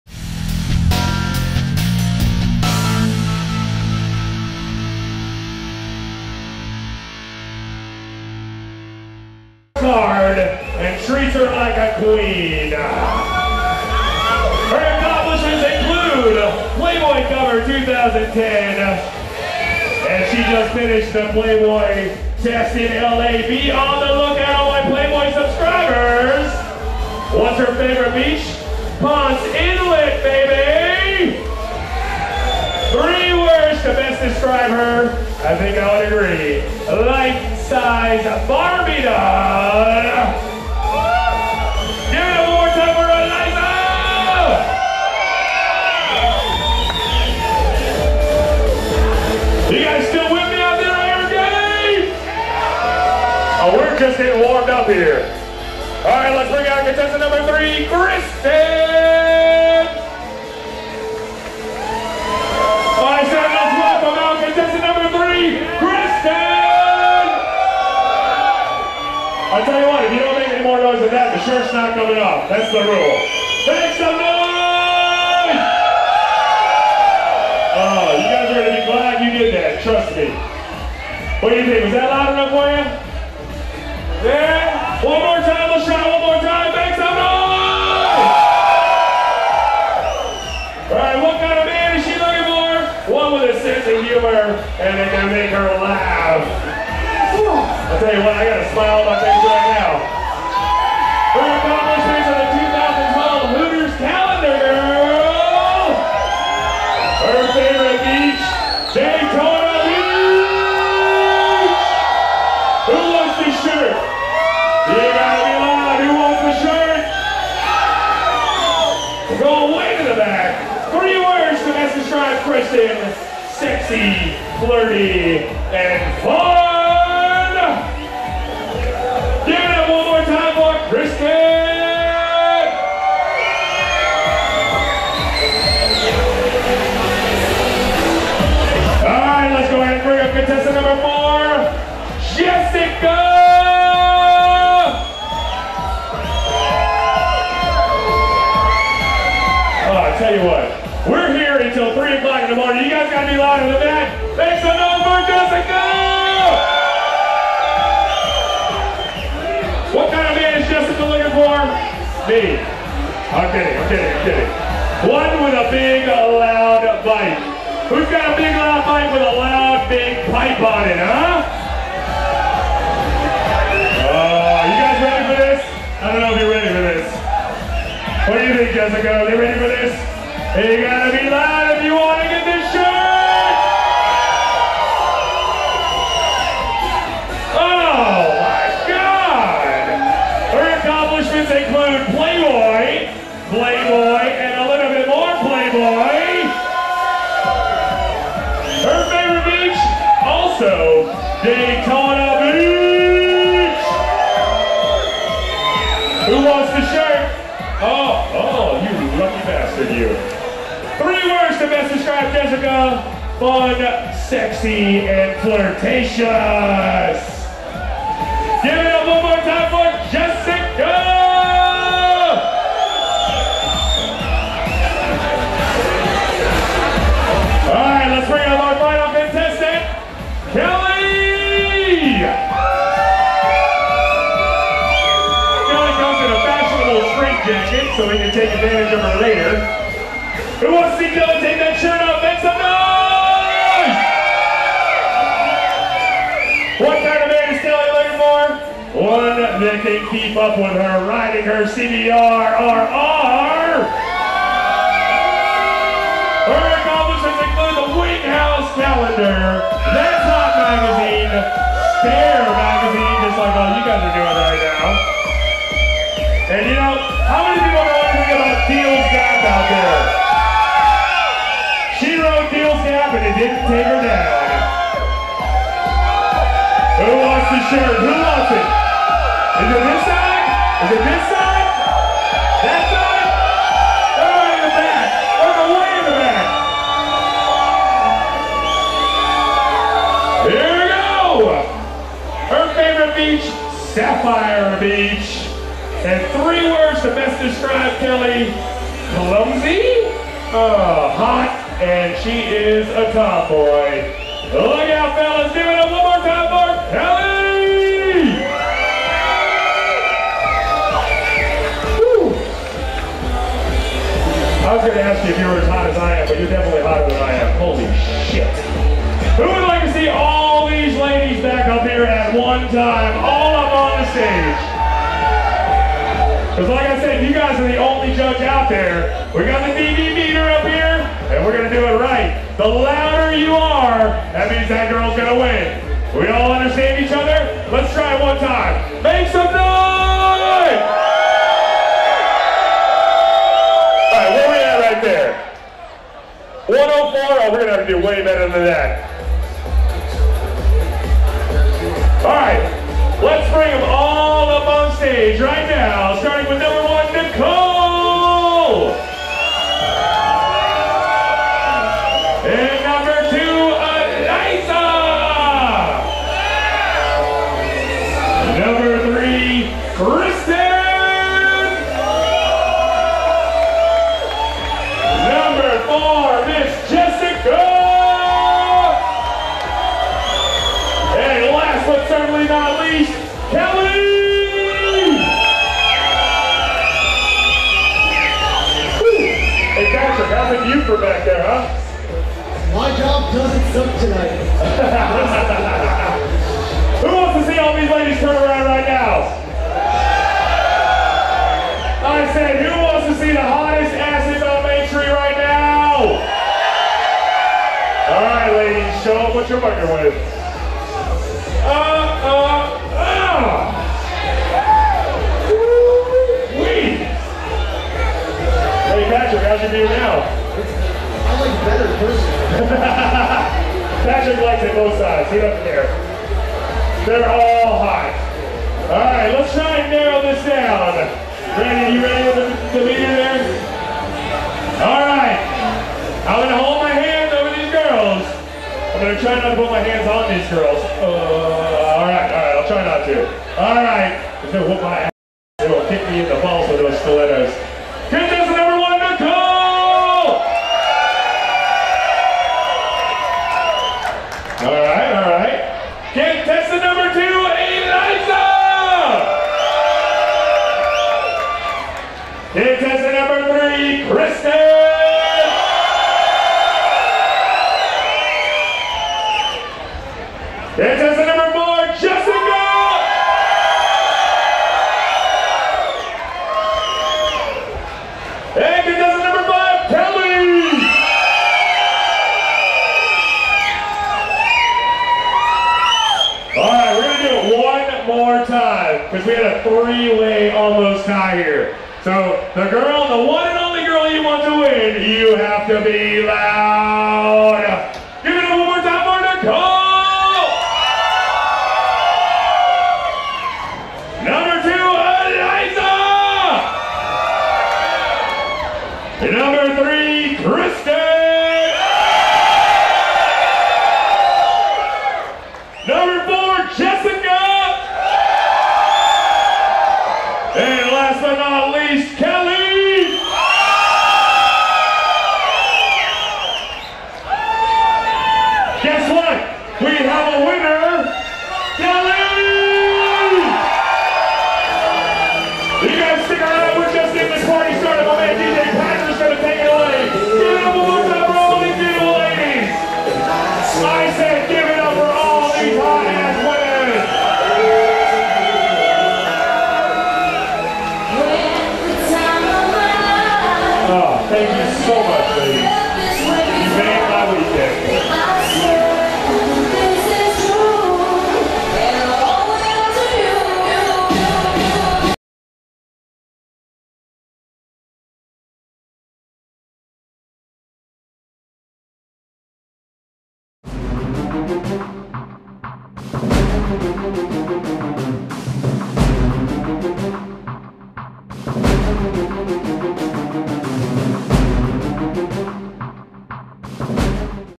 Biketoberfest 2011 Saturday Bikini Contest Part 2
Saturday night bikini contest at Biketoberfest 2011 in Daytona Beach, FL.
On the Jagermeister Stage in the Iron Gate Saloon.
Video by Pat 'Nitro' Miller-CycleShotz!
Copyright Nitros Digital LLC